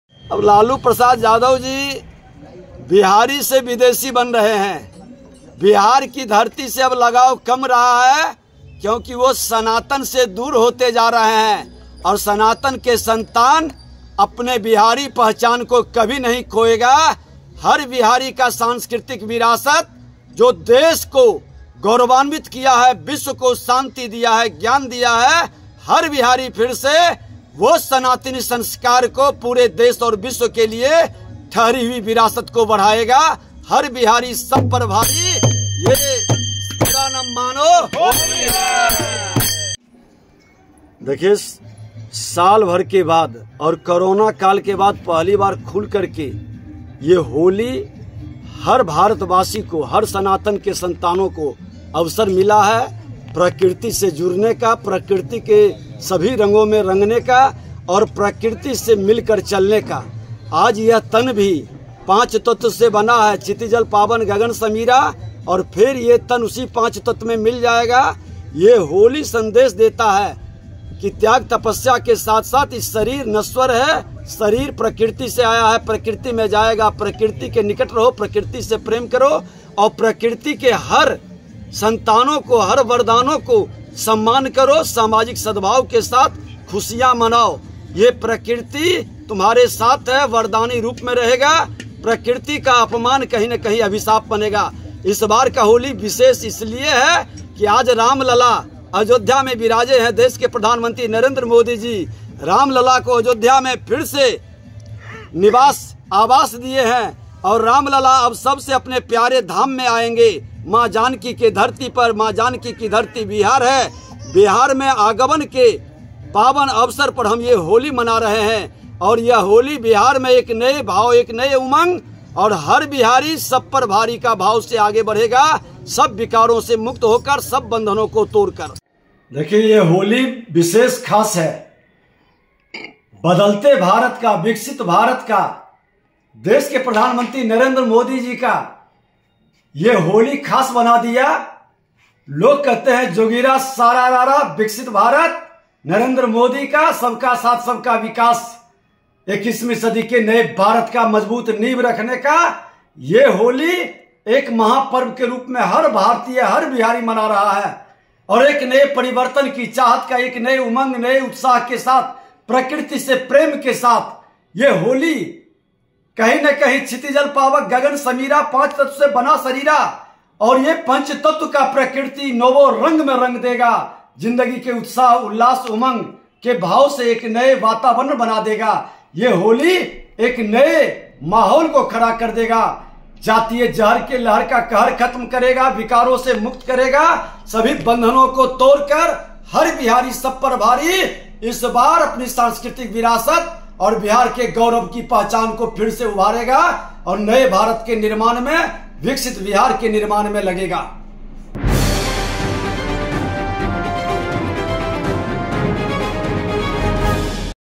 उप मुख्यमंत्री विजय सिन्हा ने खेली होली, ढोल बजाकर गाया फगुआ गीत DTW 24 NEWS
उप मुख्यमंत्री विजय सिन्हा ने खेली होली, ढोल बजाकर गाया फगुआ गीत